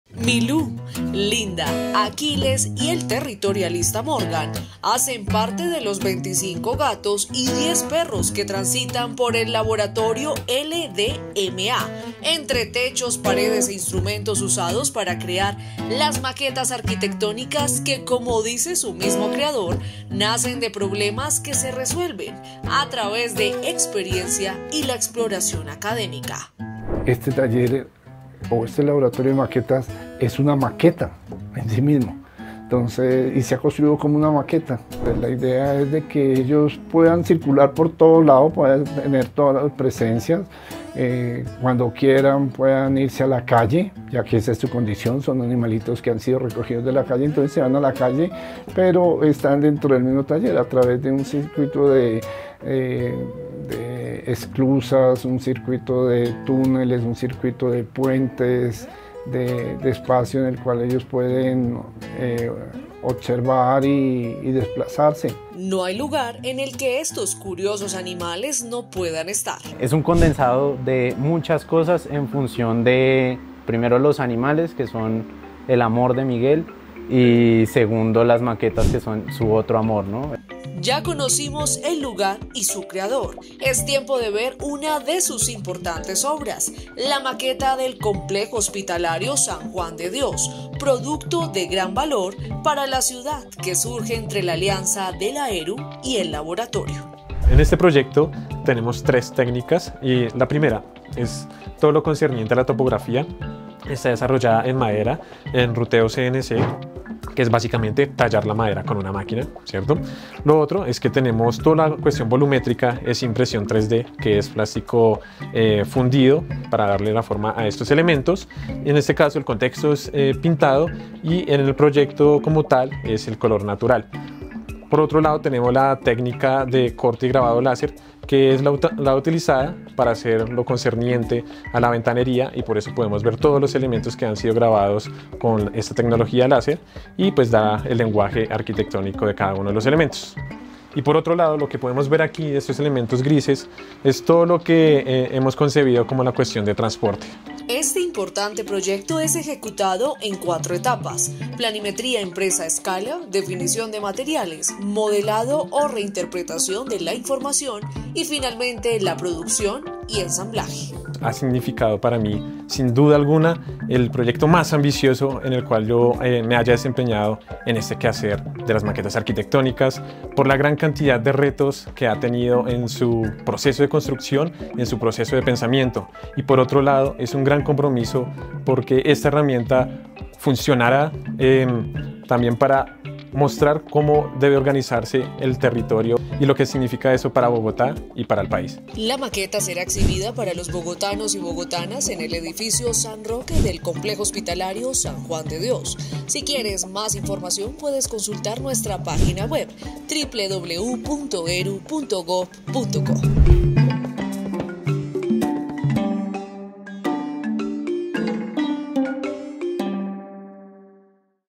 🐶🐱El laboratorio LdMA es un escenario mágico de creatividad e inspiración.
En este lugar conviven profesionales de la arquitectura y el diseño con más de 20 mascotas. Actualmente, elaboran la maqueta de ElSanJuanQueSoñamos.